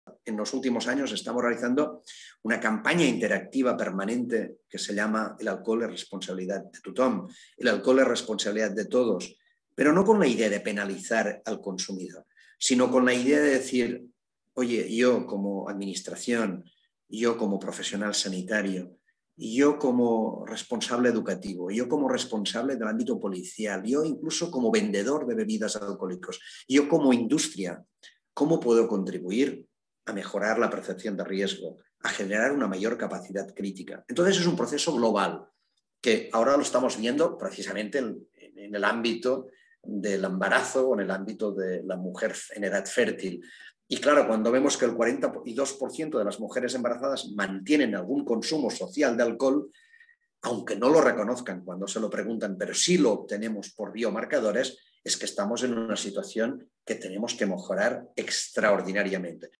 La prevención de los TEAF es responsabilidad de todos - Joan Colom
Joan Colom, subdirector general de Drogodependencias del Departament de Salut de la Generalitat de Catalunya, explica cómo se trabajan las campañas de prevención del consumo de alcohol, especialmente en las mujeres embarazadas para evitar la aparición de un trastorno del espectro alcohólico fetal (TEAF) en sus bebés.